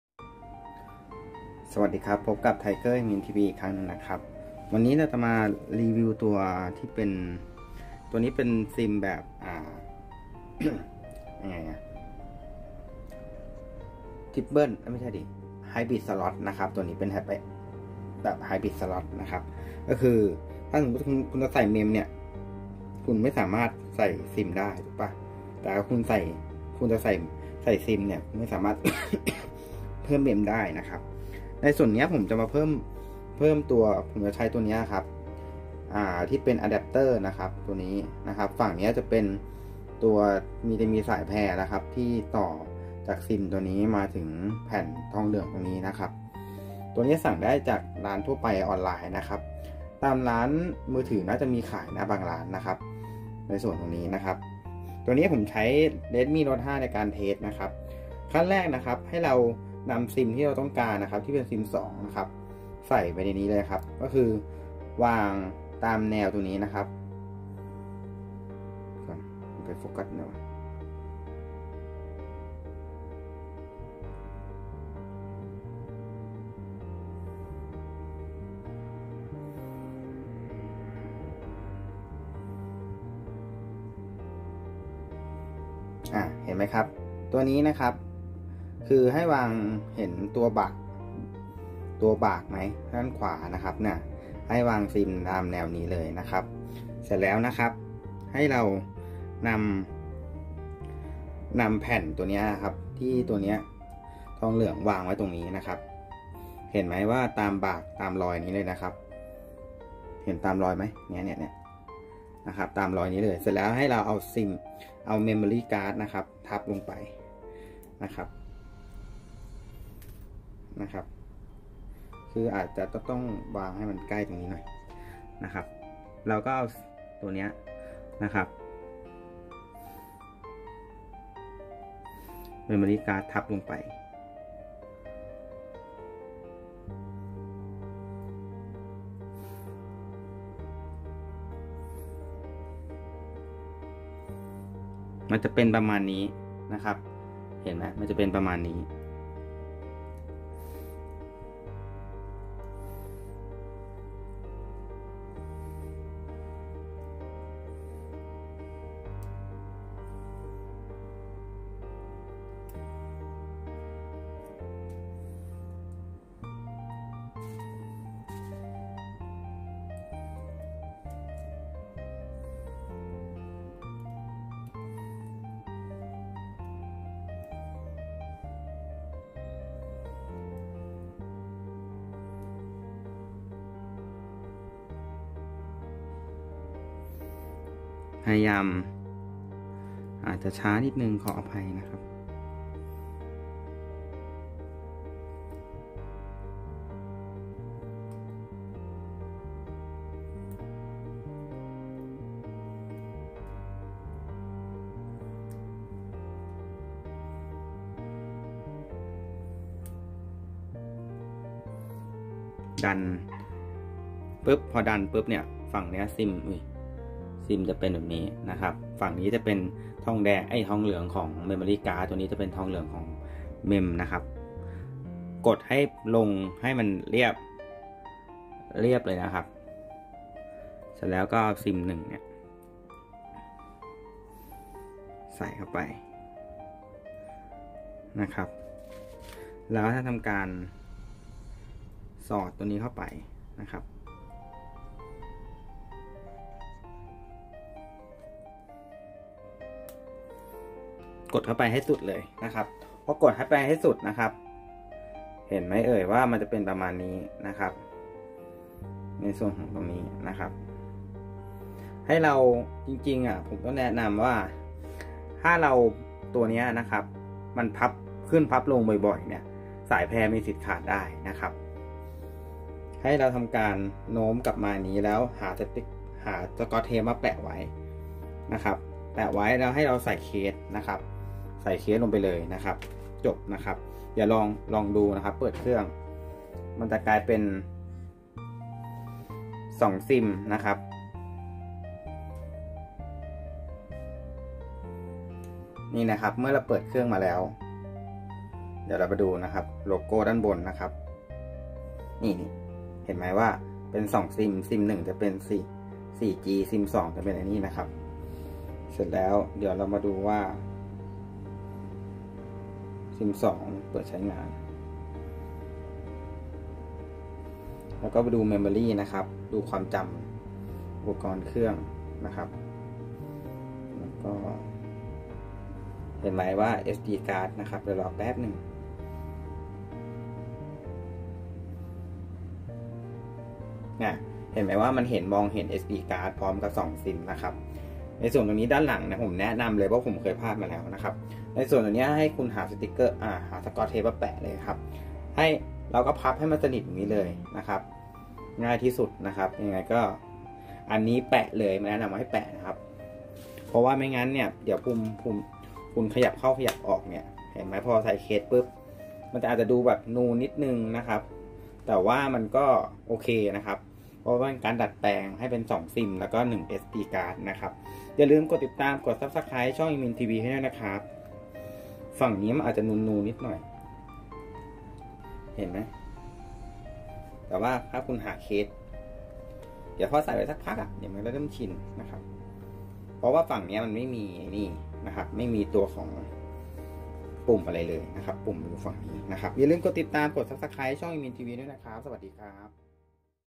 วิธีแปลง Hybrid Shot ให้ใช้ได้2ซิม และ Mirco SDได้พร้อมกัน [AminTV]
วิธีแปลง HybridShotให้ใช้ได้2ซิม และ